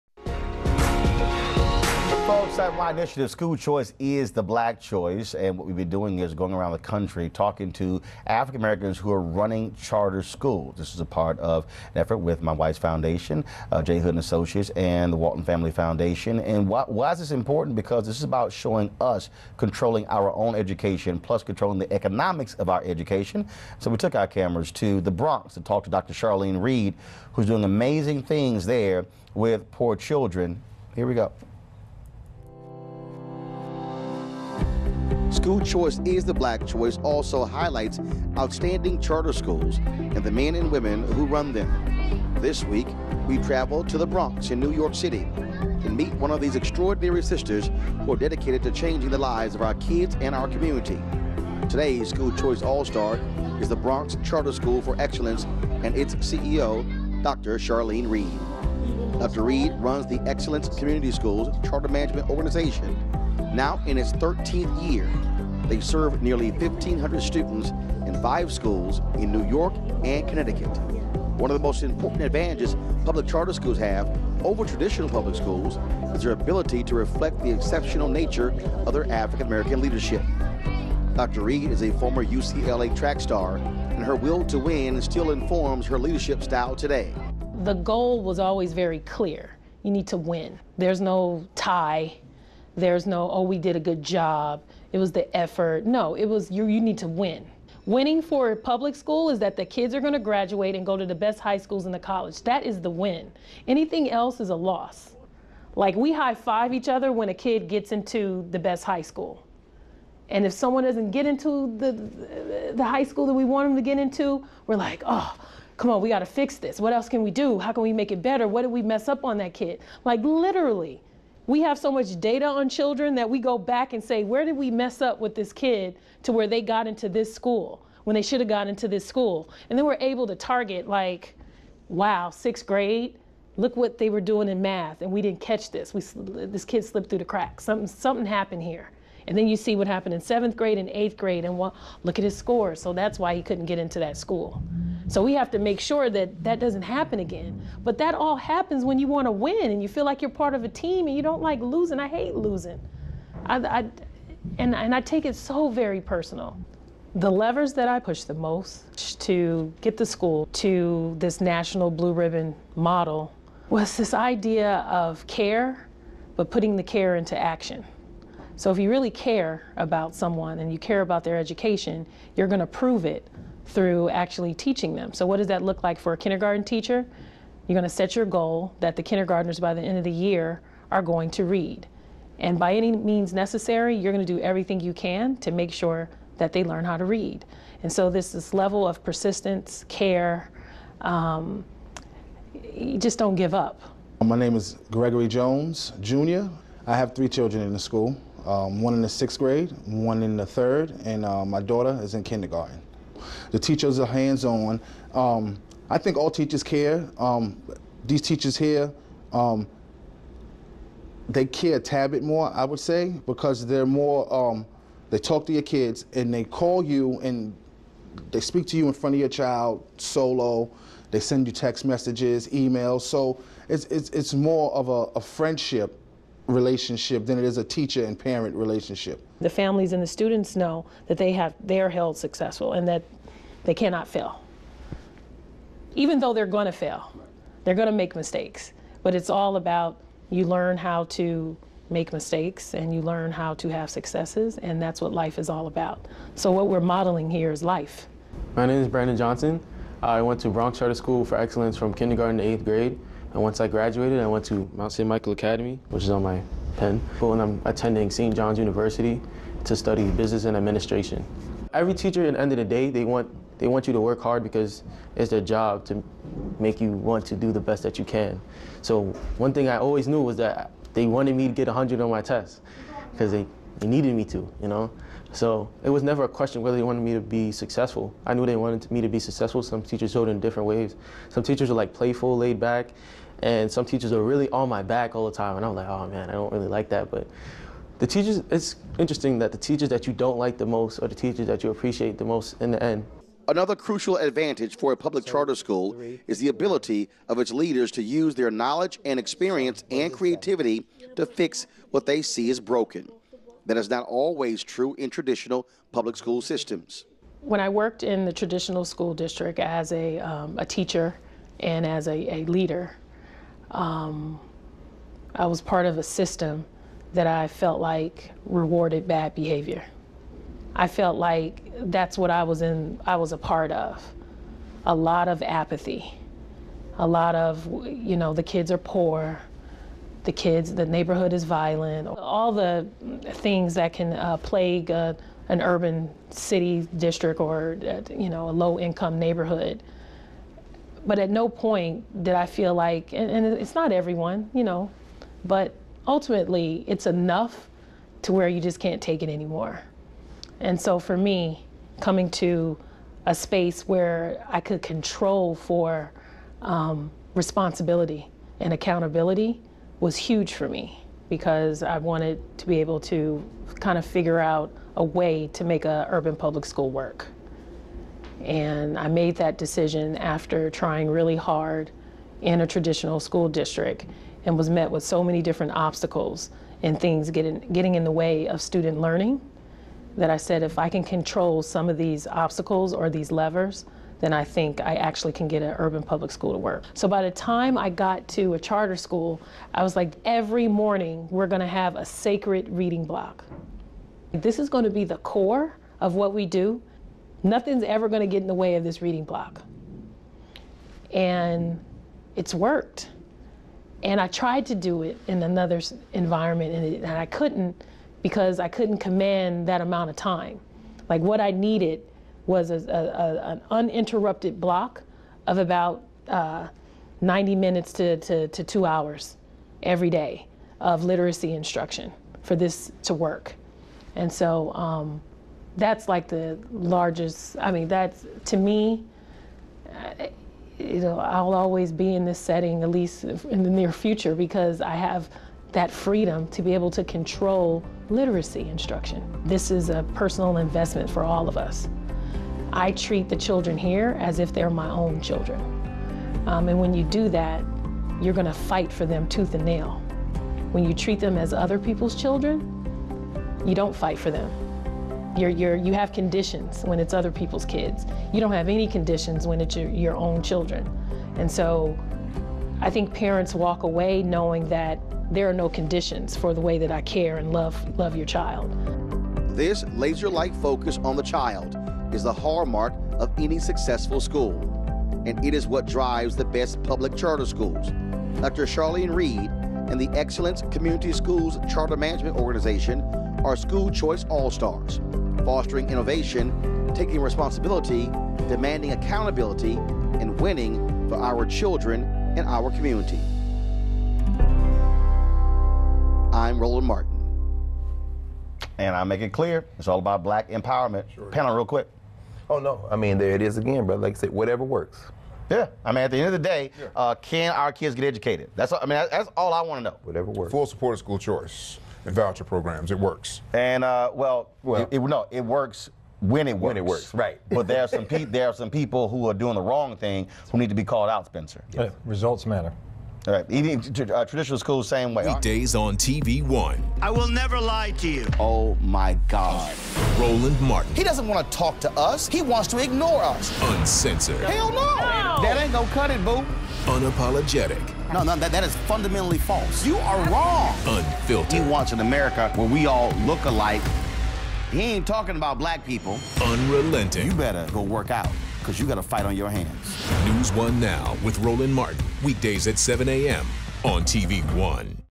School Choice All-Stars: Dr. Charlene Reid, CEO Of The Excellence Community Schools In The Bronx, NY
Dr. Charlene Reid, a former track star who insists on victory for her students and wins is the focus of this week’s installment of School Choice All-Stars.

Dr. Reid is the CEO of Excellence Community Schools in the Bronx, NY. In 2012, during her tenure as Principal of Bronx Charter School for Excellence, the institution was selected as a National Blue Ribbon School.